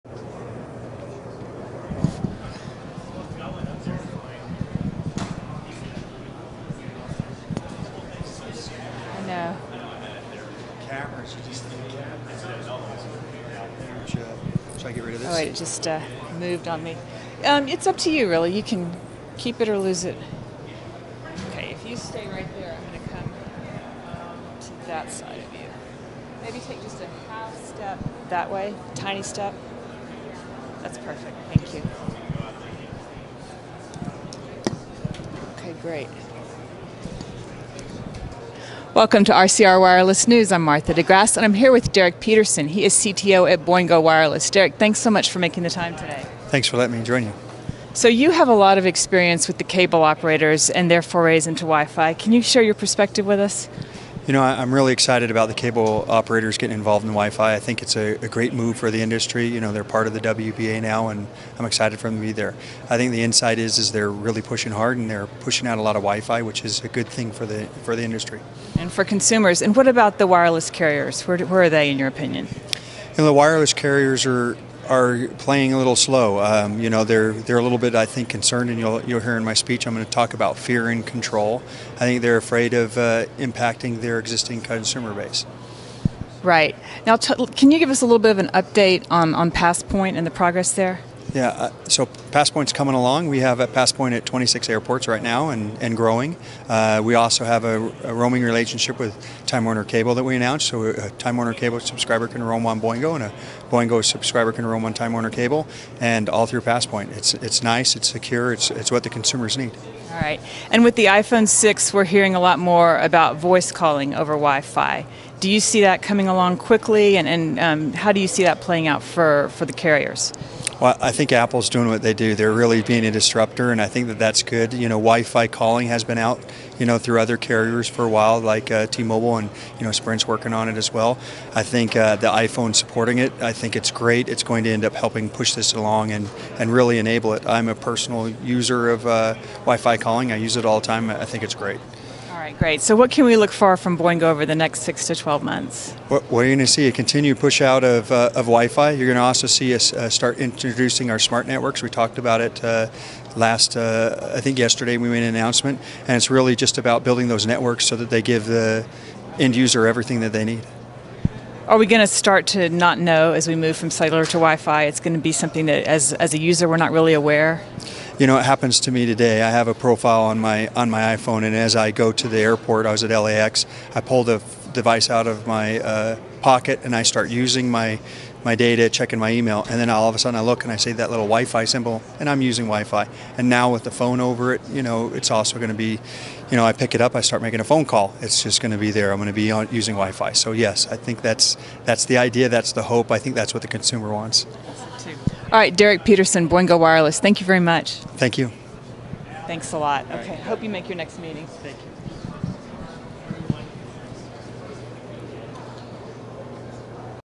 Boingo Wireless talks WiFi Calling at CTIA SuperMobility Week
Martha interviews Derek Peterson (CTO) of Boingo Wireless at CTIA SuperMobility Week. Derek gives his take on the Cable Operators and Wireless Carriers getting involved in WiFi. He also updates us on PassPoint, WiFi Calling with the IPhone6, and the future of Boingo.

MAH01607